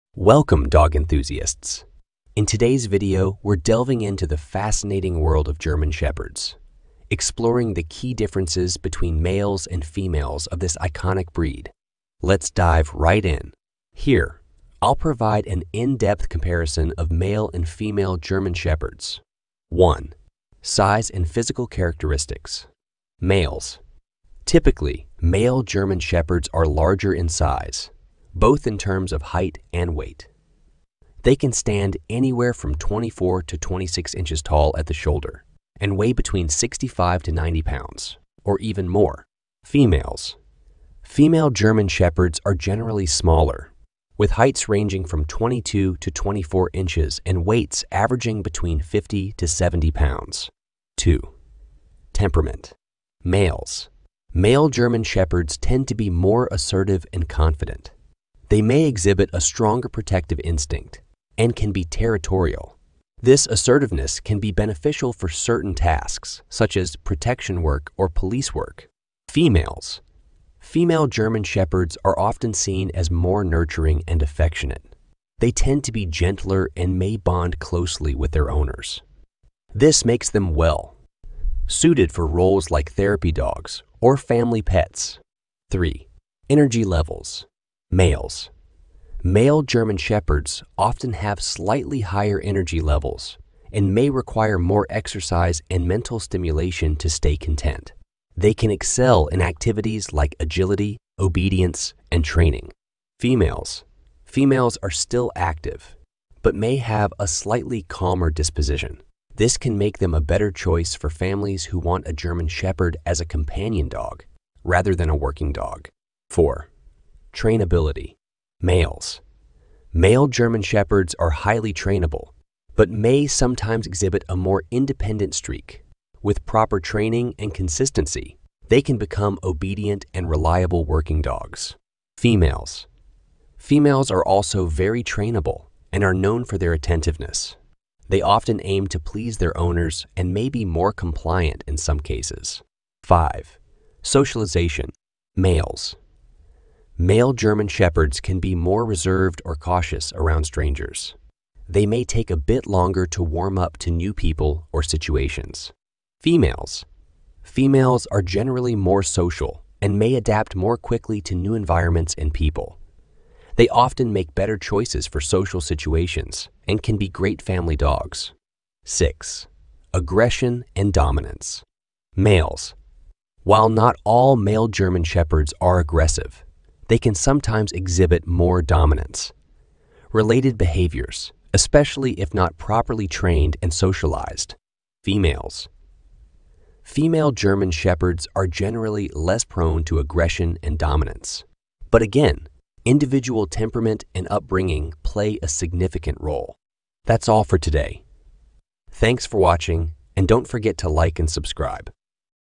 Gender Matters: German Shepherd Dog Differences - Male vs Female
🐾 Welcome to Bark2Basic! 🐾

Are you considering bringing a German Shepherd into your life? Understanding the distinctions between males and females can be crucial in making the right choice for your family. In this informative video, we'll delve into the world of German Shepherds and explore the key differences between male and female dogs.

🌟 Topics Covered:

🐶 Male vs. Female Size and Weight
🌈 Temperament Differences
💲 Price Considerations
💑 Which is Better for You: Male or Female?
🐕 Insights into German Shepherd Dog Behavior
Whether you're a seasoned dog owner or a first-time puppy parent, these insights will help you make an informed decision. Watch to the end for valuable tips and considerations when choosing the gender of your German Shepherd.